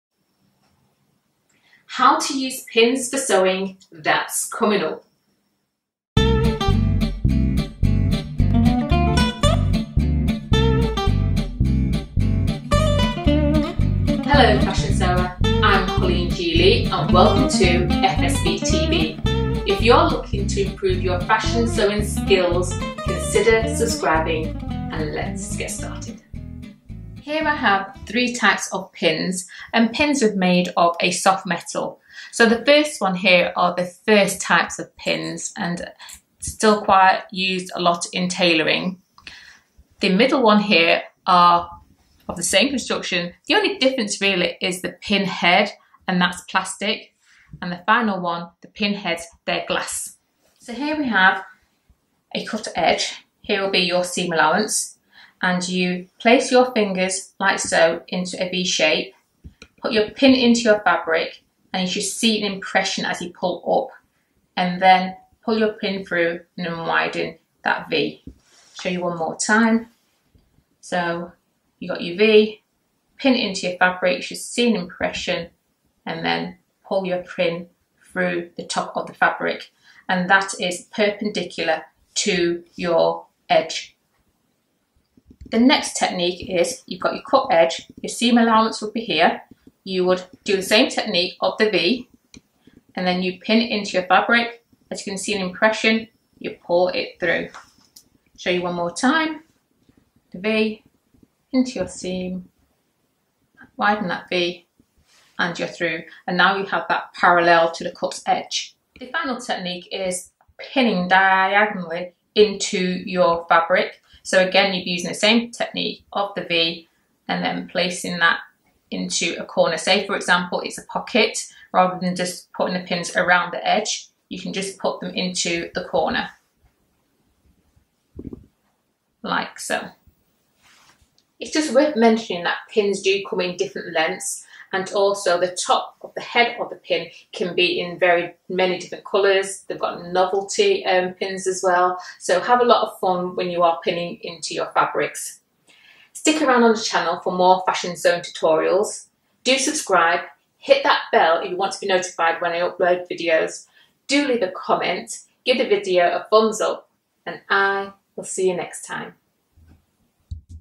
How to Use Pins for Sewing
How to Use Pins for Sewing. The straight pin is of a soft strong metal construction as a point on one end and a head at the other, which can be any from a colourful round ball to novelty heads. In the video I describe how to use them correctly and safely including there importance in fashion sewing projects